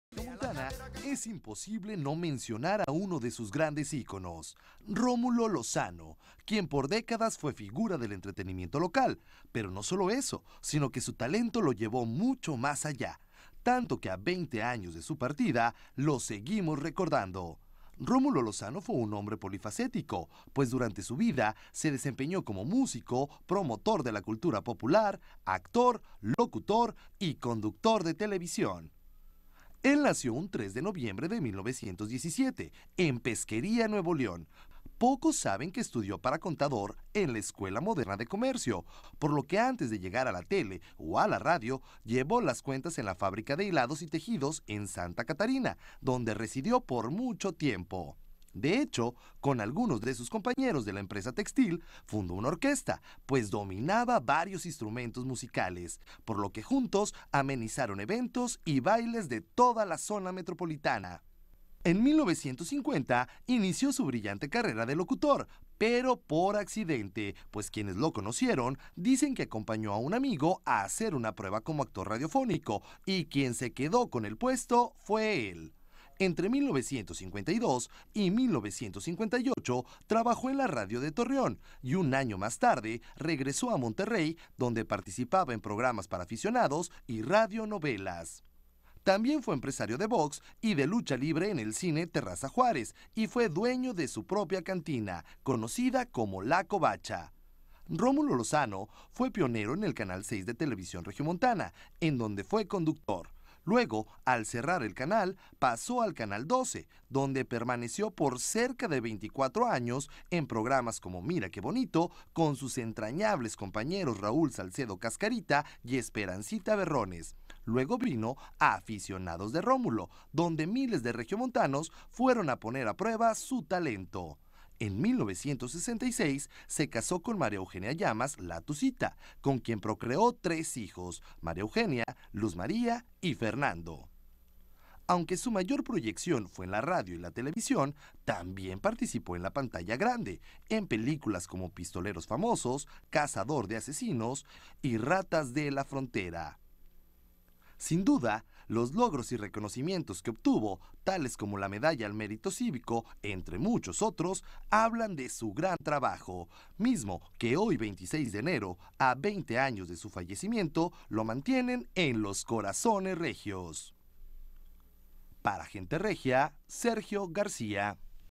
Gente Regia - Recordando a Rómulo Lozano
Reportaje especial sobre la vida de Rómulo Lozano.